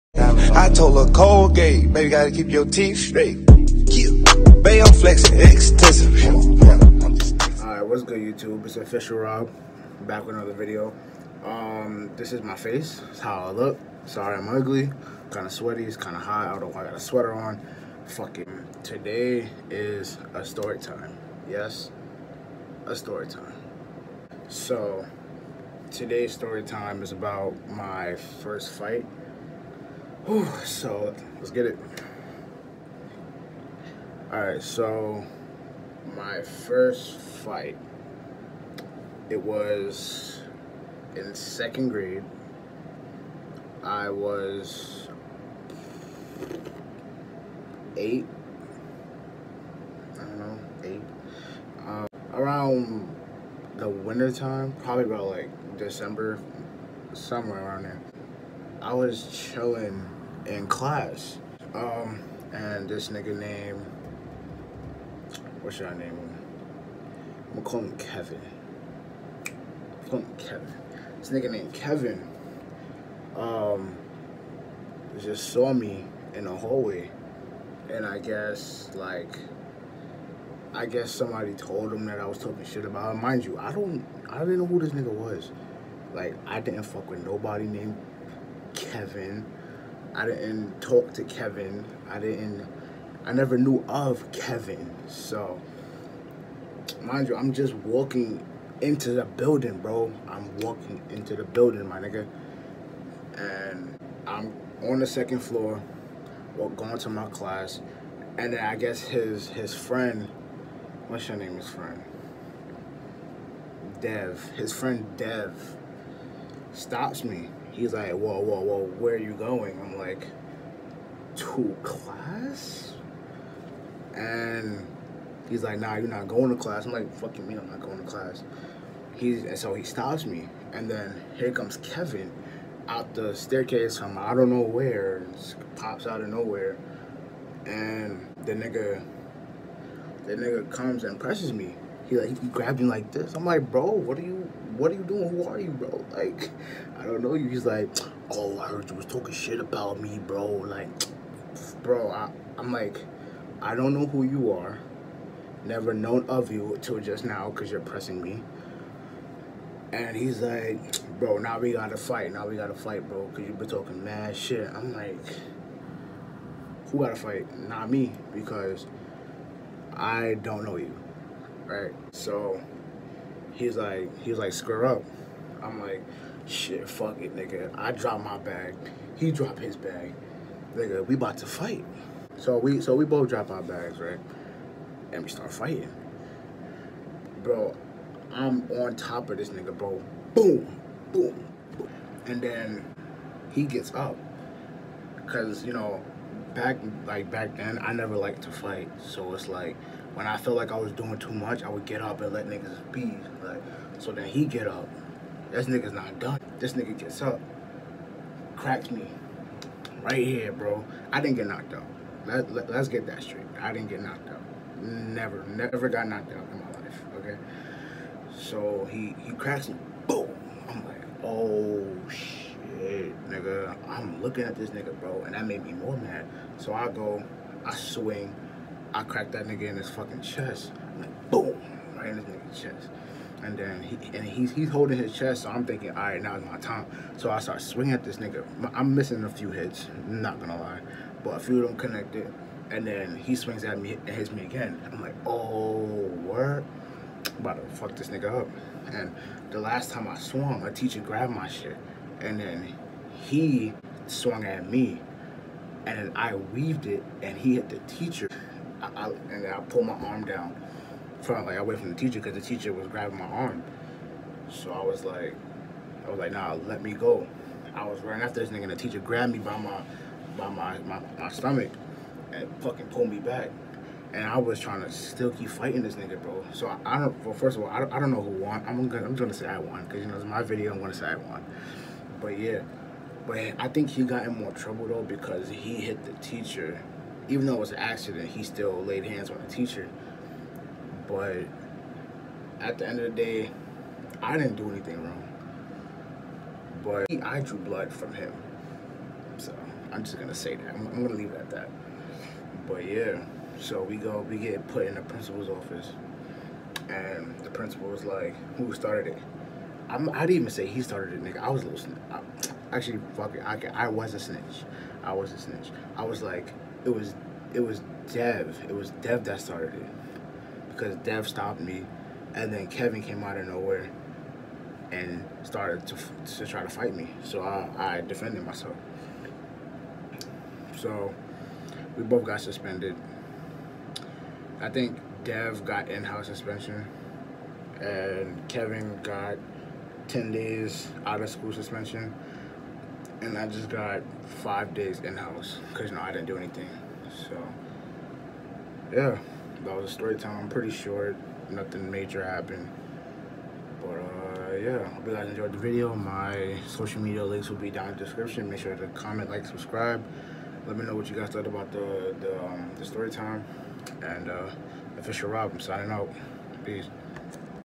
STORYTIME: MY FIRST FIGHT ‼️🤷🏽‍♂️
This is a StoryTime about my first fight I hope you guys enjoy.

Add me on Snapchat @rjgoromi